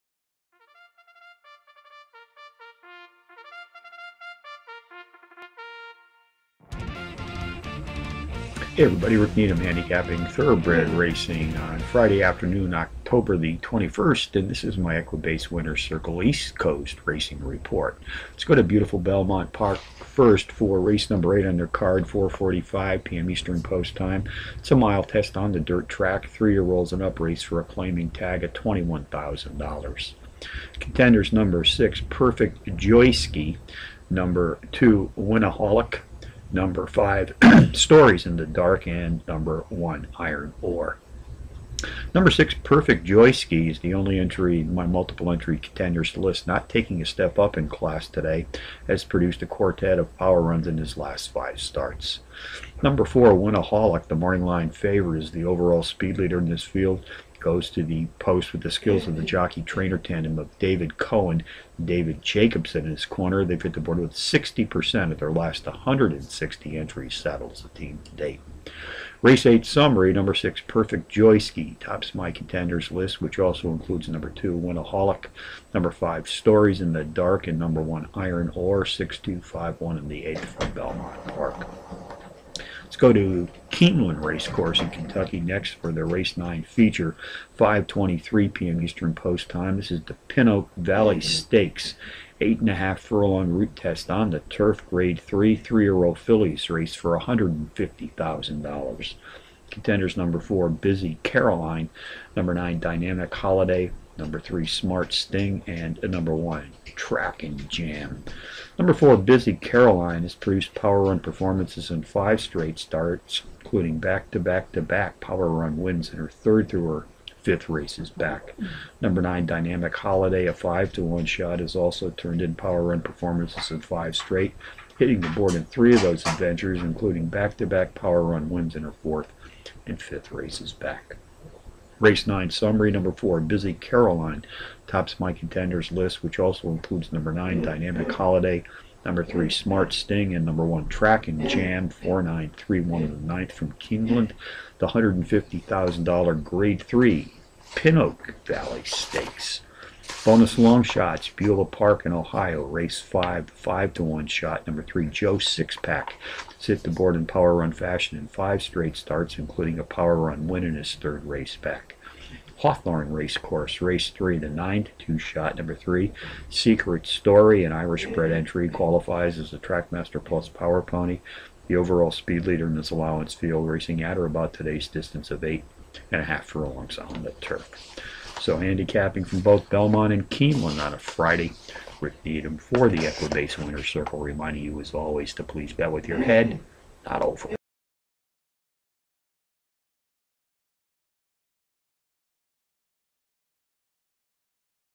East Coast Racing Report - Belmont - Keeneland - 10/21/2011
Equibase Winner's Circle - East Coast Racing Report - Belmont Park - Keeneland Race Course - Friday October 21, 2011 + bonus longshots